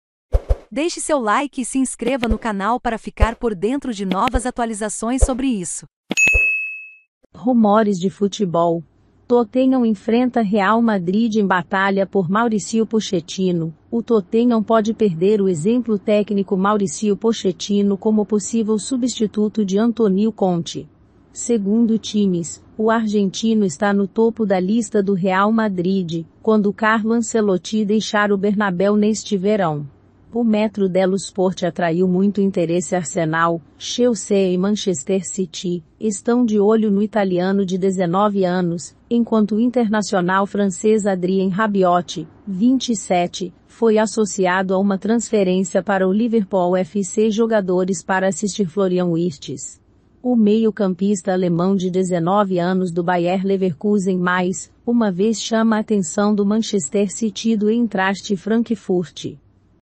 RUMORES DE FUTEBOL: TOTTENHAM ENFRENTA REAL MADRID EM BATALHA POR MAURICIO POCHETTINO
O Tottenham pode perder o ex técnico Mauricio Pochettino como substituto potencial de Antonio Conte.